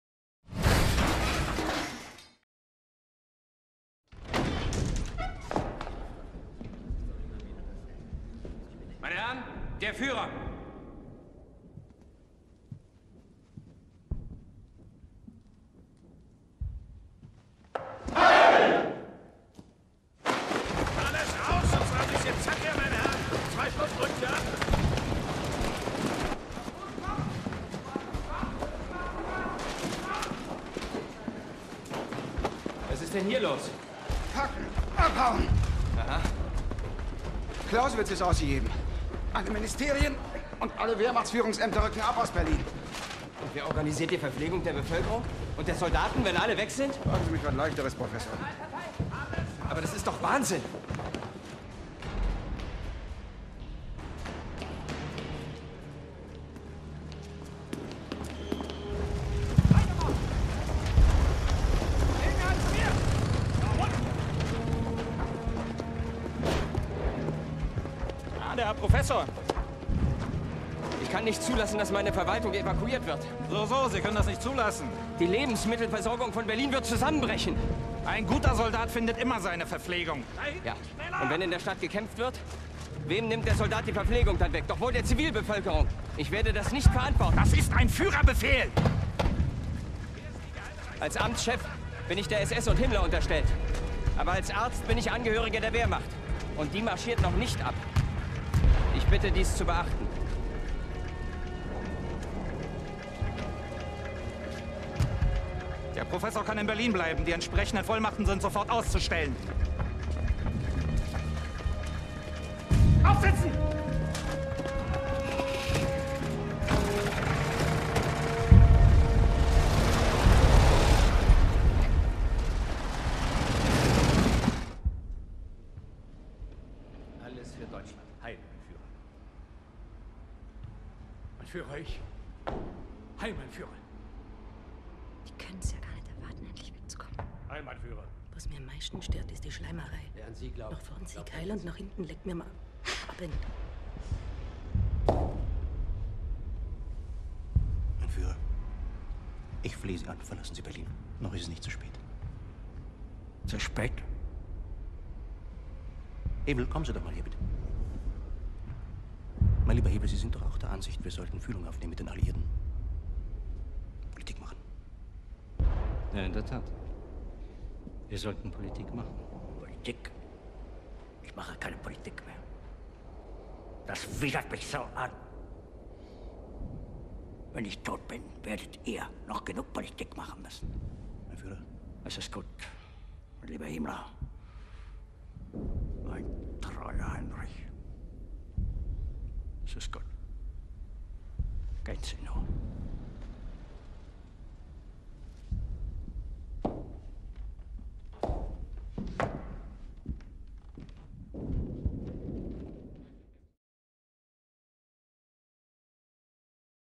Hitler: Der Auditor
Hitler inspects the system log and VLAN configuration, as the system crumbles around him.